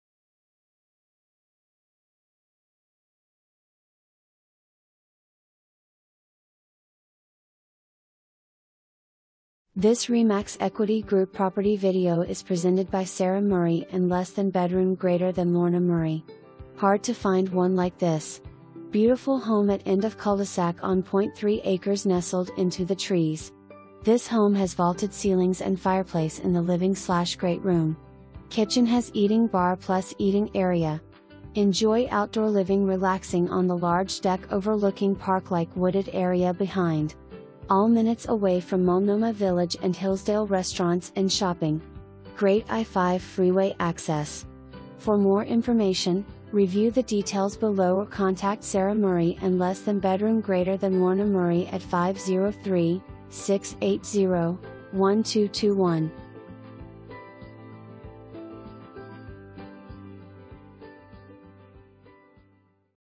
RE/MAX equity group - 2711 SW CAROLINA ST
Please enjoy this property video created by RE/MAX equity group

Hard to find one like this! Beautiful home at end of culdesac on .3 Acres nestled into the trees. This home has Vaulted ceilings and Fireplace in the Living/Great room. Kitchen has eating bar plus eating area. Enjoy outdoor living relaxing on the large deck overlooking parklike wooded area behind. All minutes away from Multnomah Village & Hillsdale restaurants and shopping. Great I-5 Freeway access.

2 full bath, 1 half bath

Sarah Murray & Lorna Murray
RE/MAX equity group